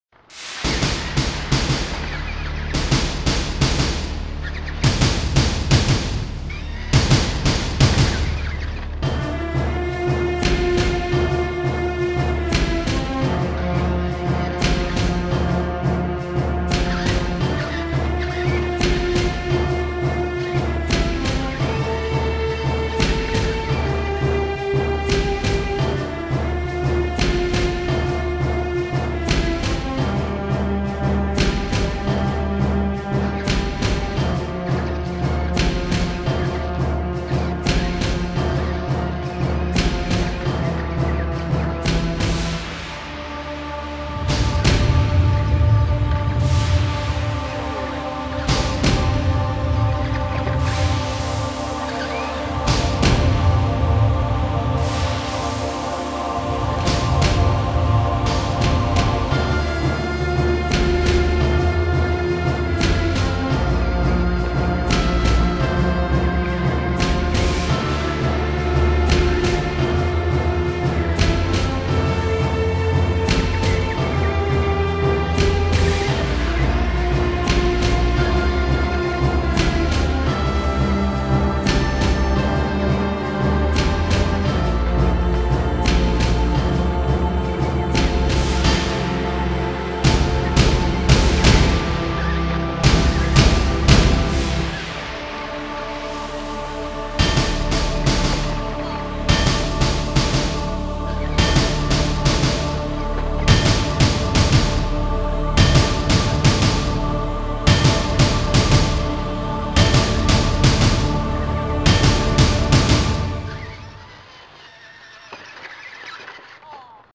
Pinata Battle
unicorn vs pony. pony remains still against vicious unicorn attack. pony wins?